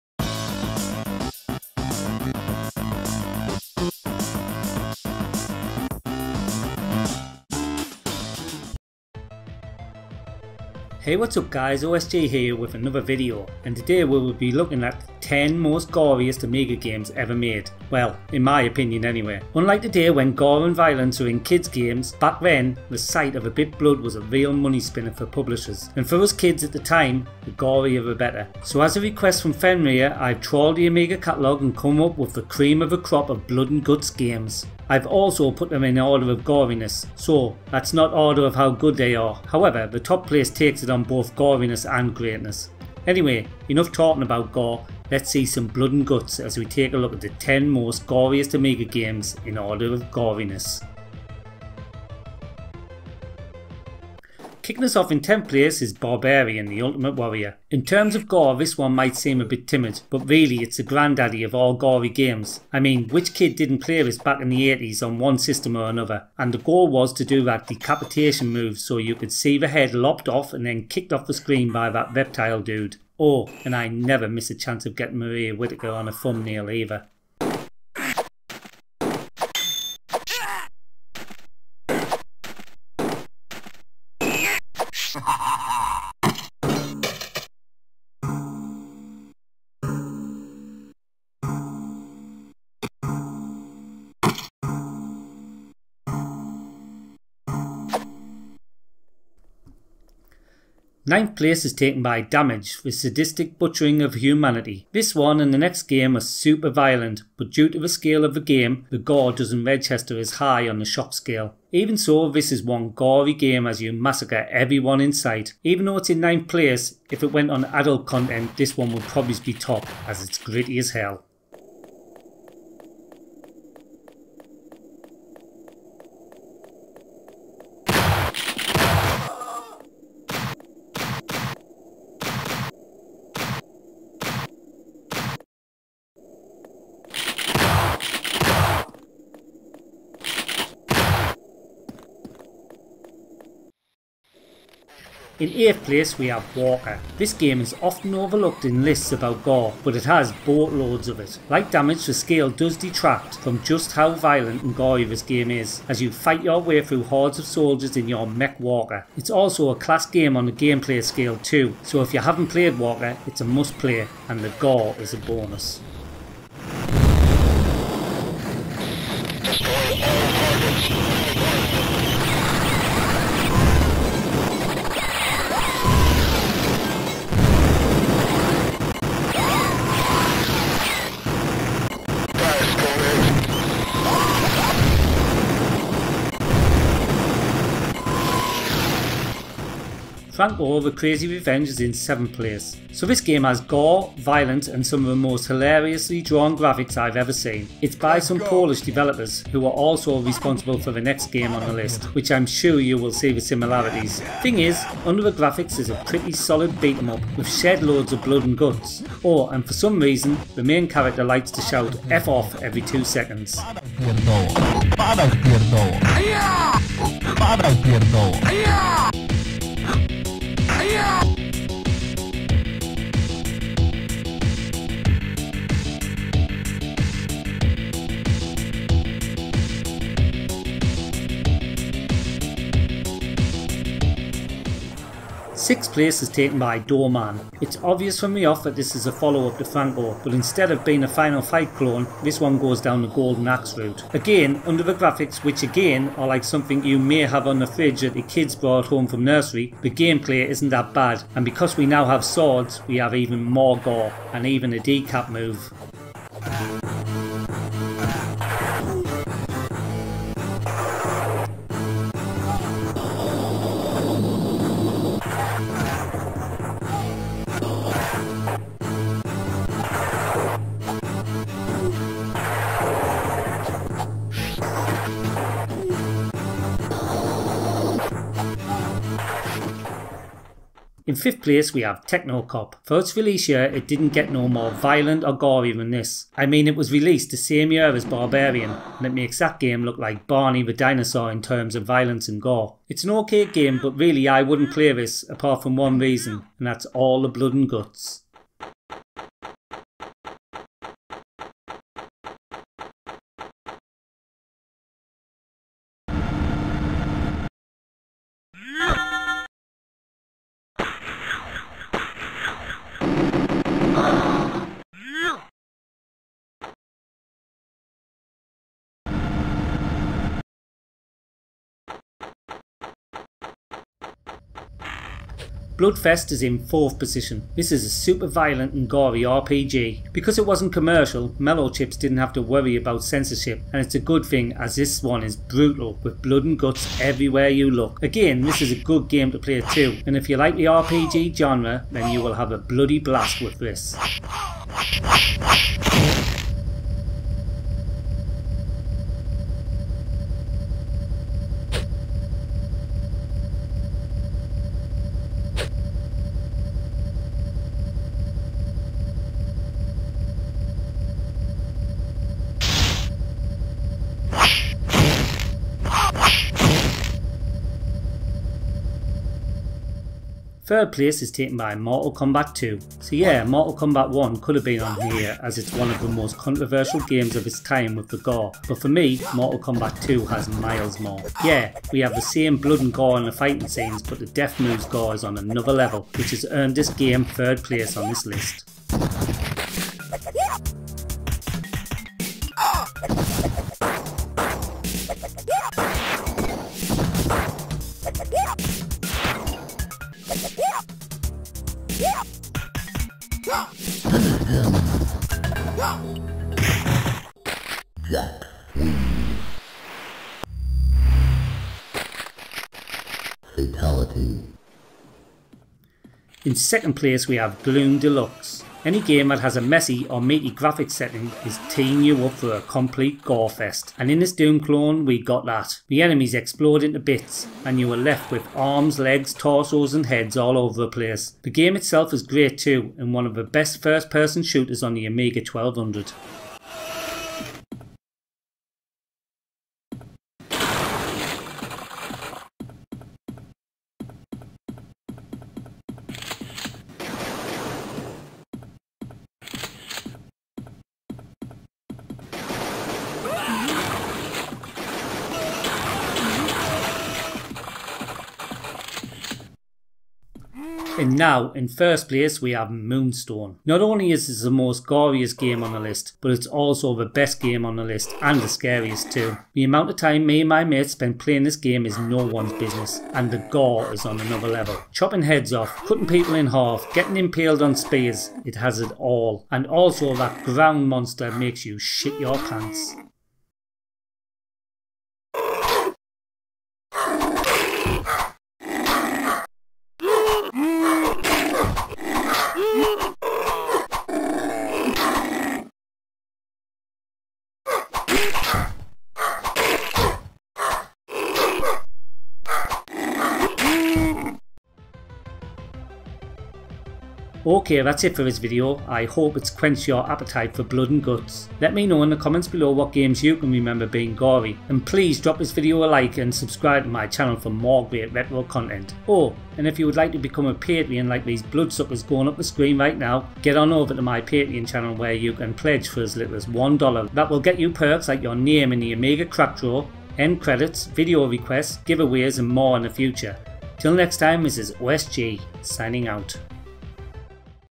The 10 Goriest Amiga Games Ever Made!
Hey whatsup guys OSG here with another video and today we will be looking at the 10 goriest Amiga games ever made…well in my opinion anyway.
Unlike today when gore and violence are in kids games, back then the sight of a bit blood was a real money spinner for publishers and for us kids at the time the gorier the better. So as a request from Fenryr. I’ve trawled the amiga catalogue and come up with the cream of the crop of blood and guts games.
I’ve also put them in order of goriness so that’s not in order of how good they are however the top place takes it on both goriness and greatness
Anyway enough talking about gore let’s see some blood and guts as we take a look at the 10 most goriest Amiga games in order of goriness